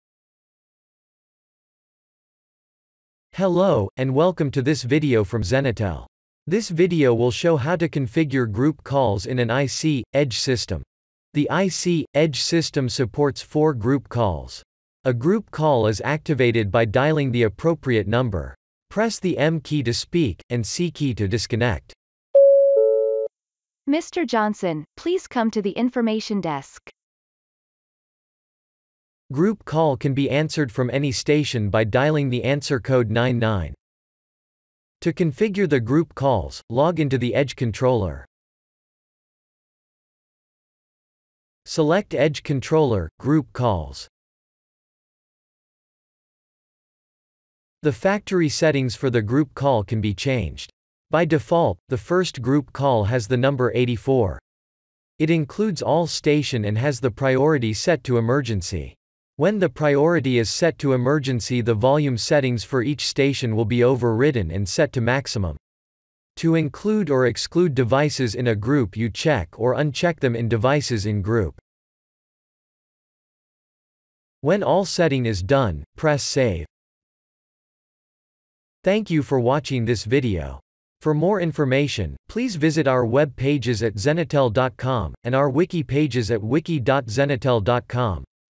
Zenitel Academy - IC-EDGE: Group Call in IC-EDGE, General
This video shows you how to configure group calls in an IC-EDGE system.